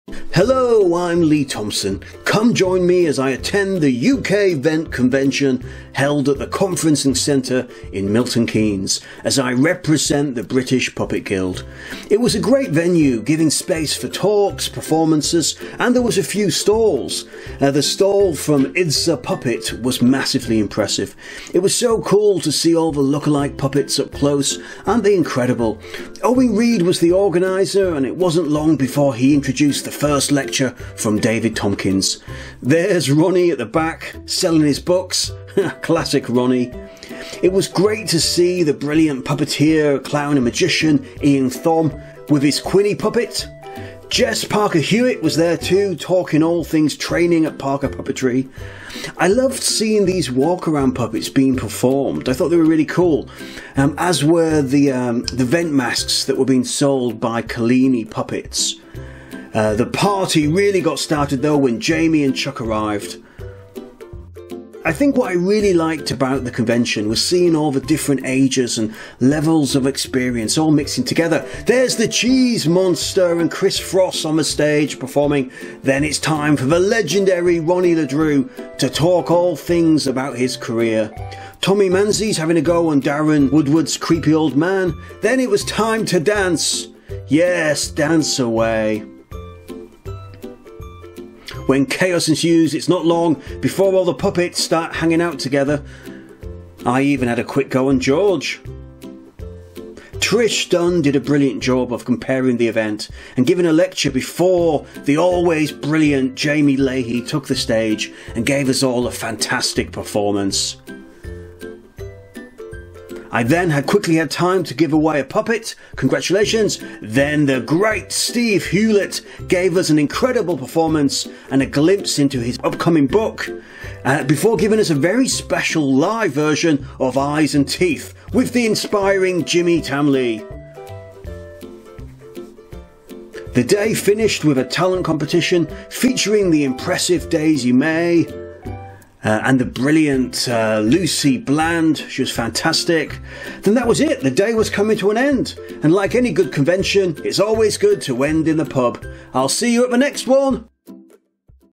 'UK Vent Convention Vlog 2024!'
Join Lee Thompson as he attends the 'U.K. Vent Convention' in Milton Keynes in June 2024.